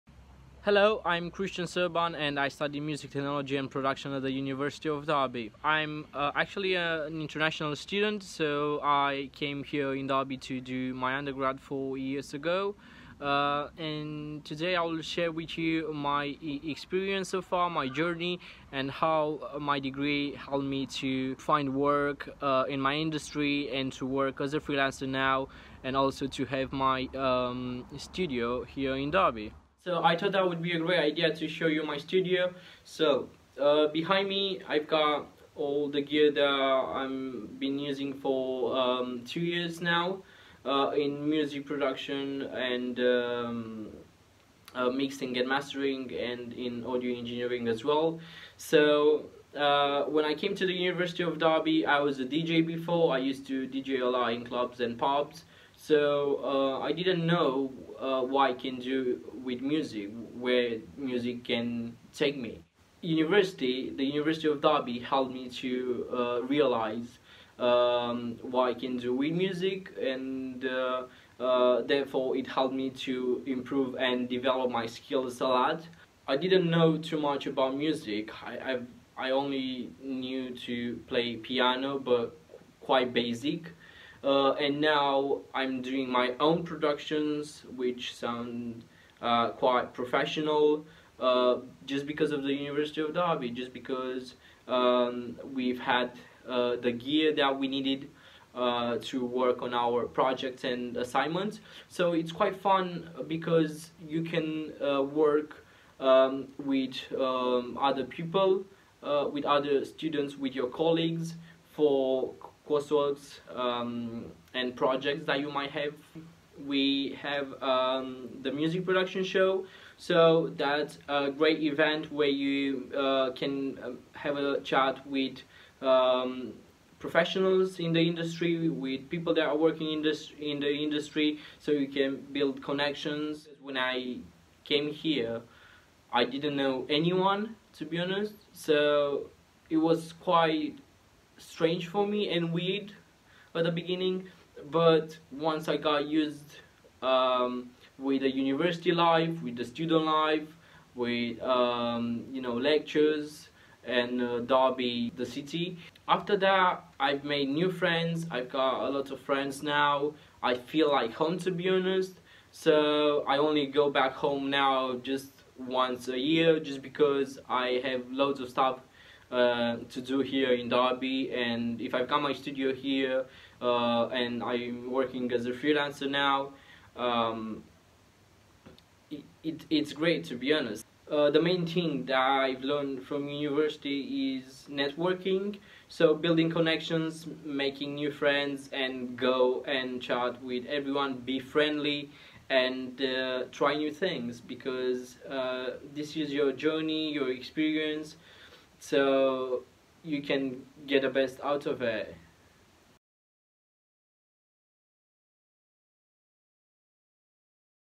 Music and Music Production at the University of Derby
Discover what it's like being a student studying Music at the University of Derby.